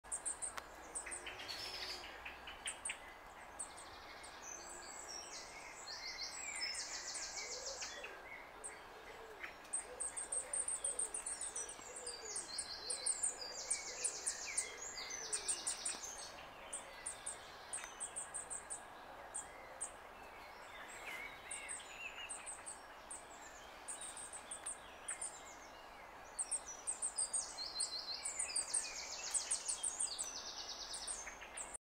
Goldcrest
A constant stream of high pitched squeaks means goldcrests may be around. The tiniest European bird, these flitty creatures are very hard to film!